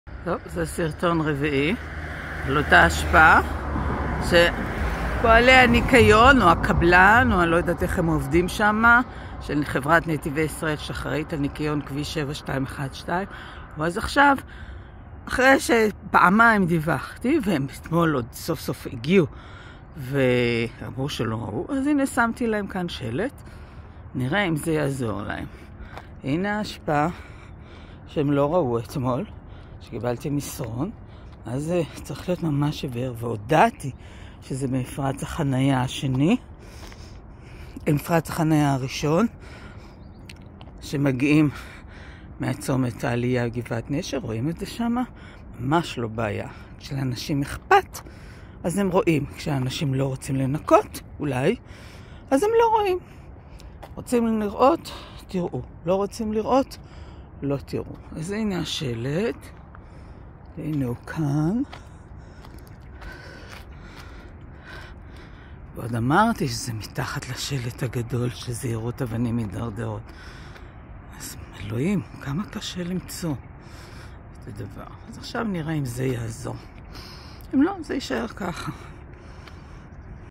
אשפה בכביש 7212 שלא פונתה ודווחתי. נתיבי ישראל הודיעו שלא מצאו את האשפה. השארתי שלט שימצאו 3/4/2023
אשפה בכביש 7212 בעיר נשר העובר בפארק הכרמל. דווח למוקד 2120* של חברת נתיבי ישראל ב-1 באפריל 2023 בבוקר. ושוב ב-2 באפריל 2023 בצהריים. ואז הם שלחו מסרון שמישהו הגיע למקום ולא מצאו. אז היום 3 באפריל 2023 לפני 7 בבוקר הגעתי למקום שמתי שלט על העמוד ודיווחתי שוב. מחר ה-4 באפריל אבדוק אם נוקה. נתיבי ישראל וסאגת הניקיון הבלתי נגמרת